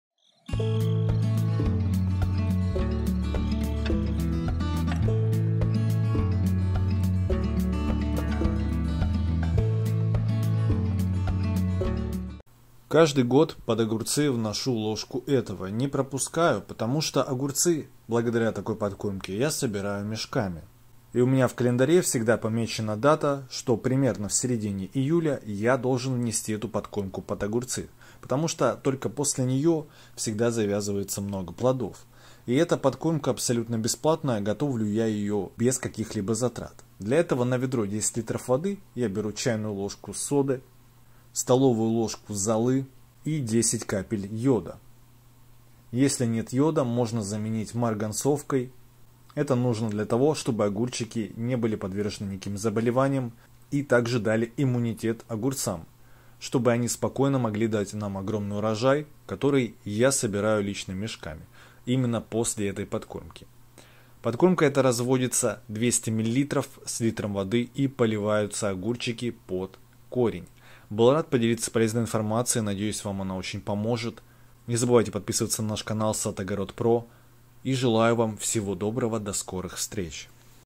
Одна ложка под огурцы и огурцы мешками собираю!
Всего 1 ложка и огурцы мешками собираю с грядки, в теплице огурцы кормлю так же этим и всегда огурцы ведрами собираю,супер подкормка огурцов своими руками для сад и огорода под огурцы. Всего одна ложка заменит все подкормки и удобрения для огурцов

1 ЛОЖКА! ЧЕРЕНКИ ВСЕ В КОРНЯХ ЗА 2 ДНЯ! МОЩНЫЙ ОТВАР ДЛЯ ОБРАЗОВАНИЯ КОРНЕЙ СВОИМИ РУКАМИ